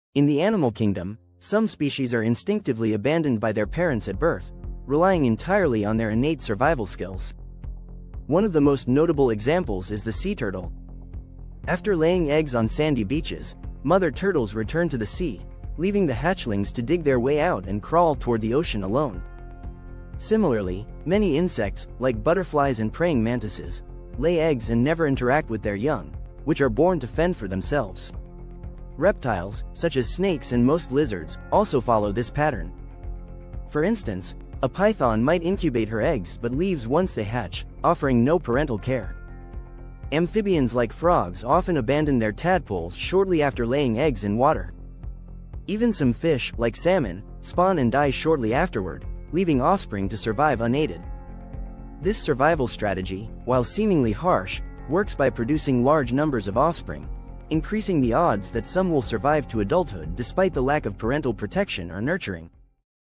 Which Species Abandon Their Young At Birth?
Did you know that some species abandon their young right after their born?